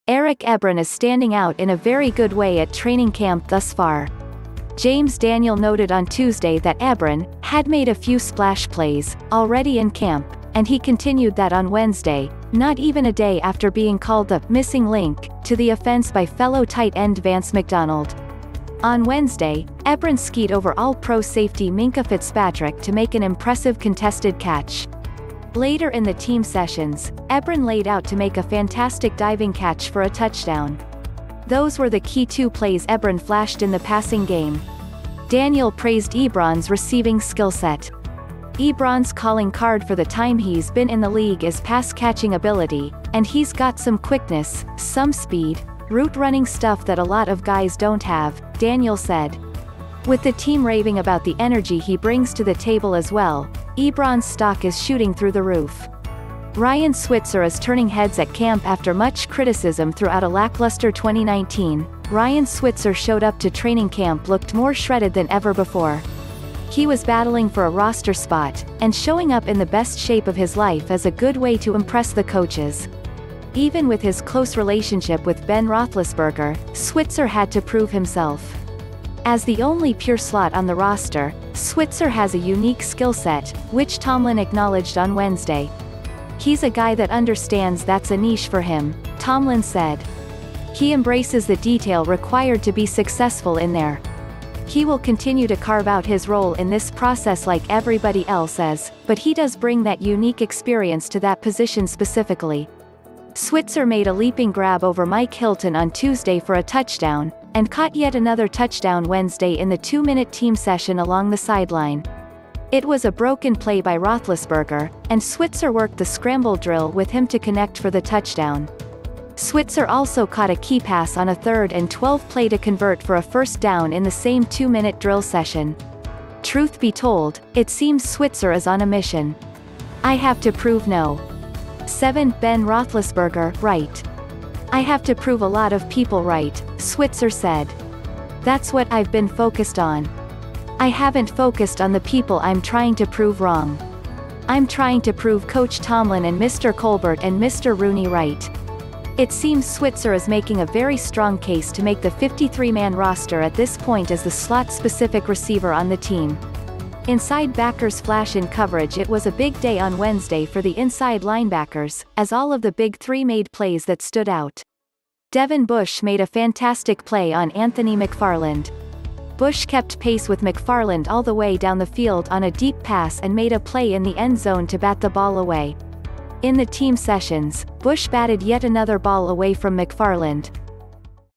Jaguars Release RB Leonard Fournette Steelers again Speculated as
Eric Ebron is standing out in a very good way at training camp thus far. James Daniel noted on Tuesday that Ebron “had made a few splash plays” already in camp, and he continued that on Wednesday, not even a day after being called the “missing link” to the offense by fellow tight end Vance McDonald. On Wednesday, Ebron skied over All-Pro safety Minkah Fitzpatrick to make an impressive contested catch. Later in the team sessions, Ebron laid out to make a fantastic diving catch for a touchdown. Those were the key two plays Ebron flashed in the passing game. Daniel praised Ebron’s receiving skillset.“Ebron’s calling card for the time he’s been in the League is pass-catching ability, and he’s got some quickness, some speed, route-running stuff that a lot of guys don’t have,” Daniel said. With the team raving about the energy he brings to the table as well, Ebron’s stock is shooting through the roof. Ryan Switzer is Turning Heads at CampAfter much criticism throughout a lackluster 2019, Ryan Switzer showed up to training camp looked more shredded than ever before. He was battling for a roster spot, and showing up in the best shape of his life is a good way to impress the coaches. Even with his close relationship with Ben Roethlisberger, Switzer had to prove himself. As the only pure slot on the roster, Switzer has a unique skill set, which Tomlin acknowledged on Wednesday.“He’s a guy that understands that’s a niche for him,” Tomlin said. “He embraces the detail required to be successful in there. He will continue to carve out his role in this process like everybody else is, but he does bring that unique experience to that position specifically.”Switzer made a leaping grab over Mike Hilton on Tuesday for a touchdown, and caught yet another touchdown Wednesday in the two-minute team session along the sideline. It was a broken play by Roethlisberger, and Switzer worked the scramble drill with him to connect for the touchdown. Switzer also caught a key pass on a 3rd and 12 play to convert for a first down in the same two-minute drill session. Truth be told, it seems Switzer is on a mission.“I have to prove No. 7 (Ben Roethlisberger) right. I have to prove a lot of people right,” Switzer said. That’s what I’ve been focused on. I haven’t focused on the people I’m trying to prove wrong. I’m trying to prove Coach Tomlin and Mr. Colbert and Mr. Rooney right.”It seems Switzer is making a very strong case to make the 53-man roster at this point as the slot specific receiver on the team. Inside backers flash in coverageIt was a big day on Wednesday for the inside linebackers, as all of the big three made plays that stood out. Devin Bush made a fantastic play on Anthony McFarland. Bush kept pace with McFarland all the way down the field on a deep pass and made a play in the end zone to bat the ball away. In the team sessions, Bush batted yet another ball away from McFarland.